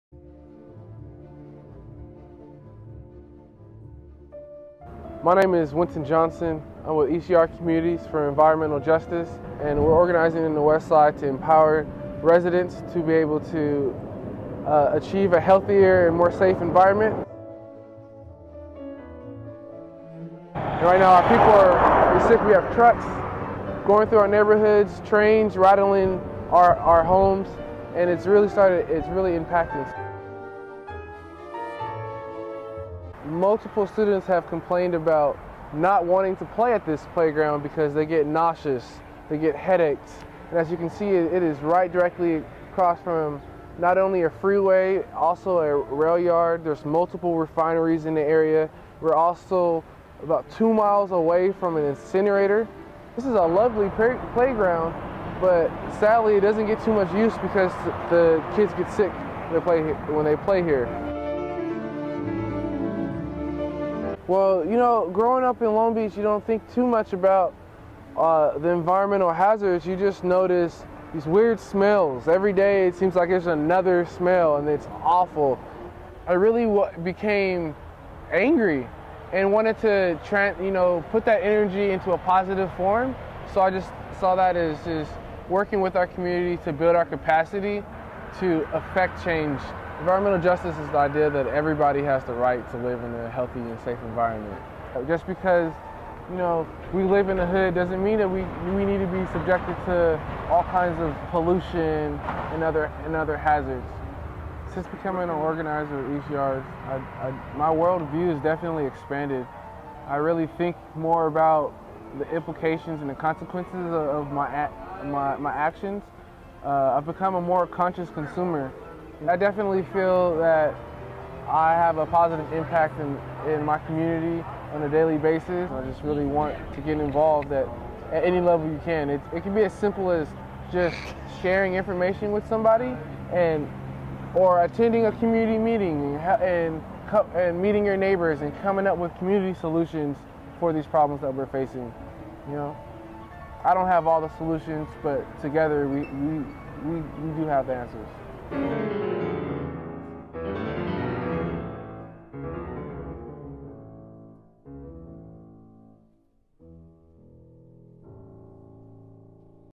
Wynton Johnson - Environmental Warrior
Wynton Johnson was born and raised in Long Beach, CA. He currently lives in West Long Beach and is studying Sociology at Long Beach City College. The correlation of where people live matters in relation to life expectancy and general health has lead him to become an environmental justice advocate. Wynton is working towards a world where health is not a luxury. He is dedicated to organizing for the empowerment of marginalized communities to advance their quality of life.